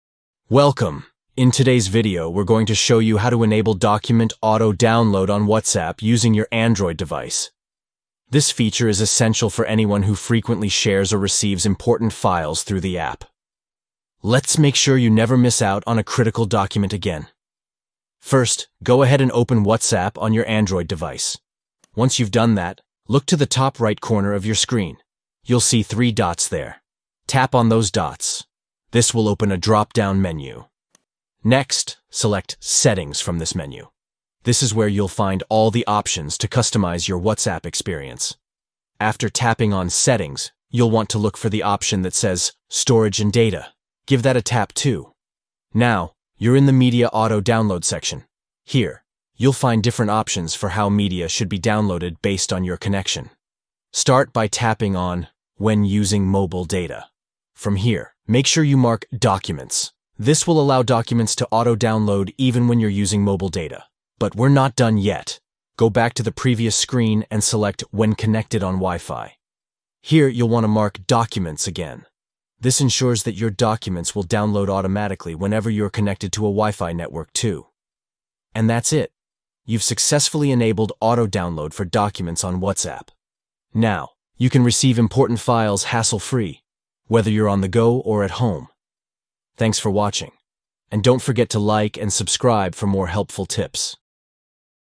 How To Enable Documents Auto-Download on WhatsApp (Updated)
Learn how to enable document auto-download on WhatsApp with this updated guide. Follow these simple steps:

1. Open WhatsApp on your Android device.
2. Tap the three dots at the top right.
3. Tap on "Settings".
4. Tap "Storage and Data".
5. Under Media auto-download section, tap "When Using mobile Data".
6. Mark "Documents".
7. Go back to "When connected on Wi-Fi" then mark "Documents".

Chapters:
00:00 Intro
00:12 Soultion
01:05 Outro